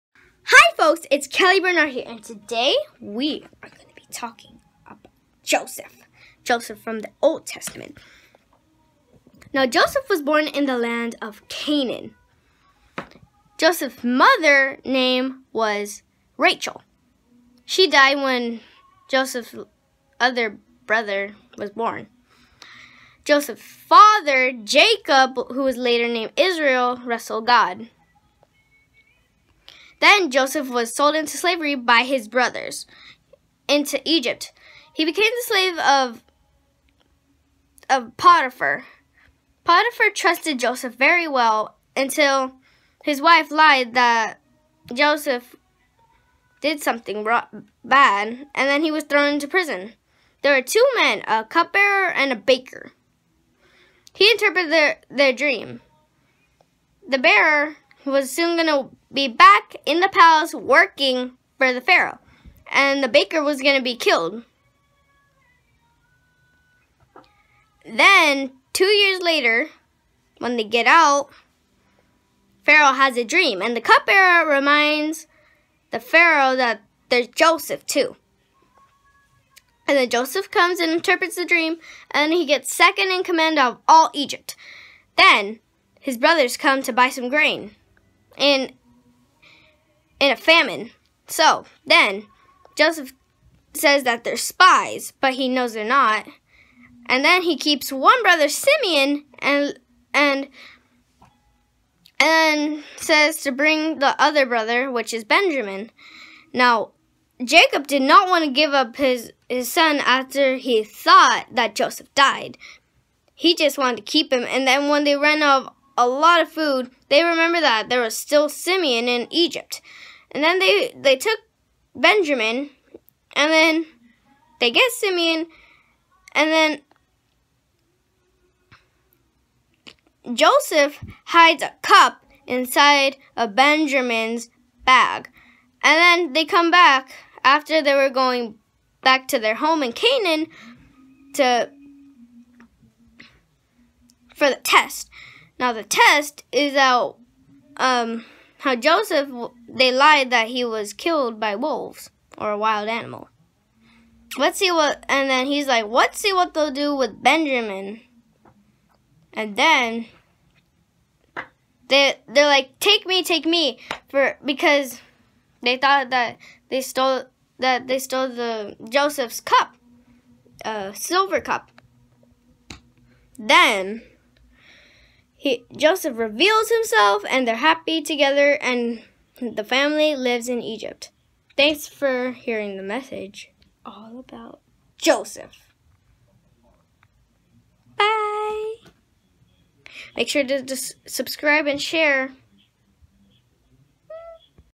Joseph
this is a short story of Joseph the son of Jacob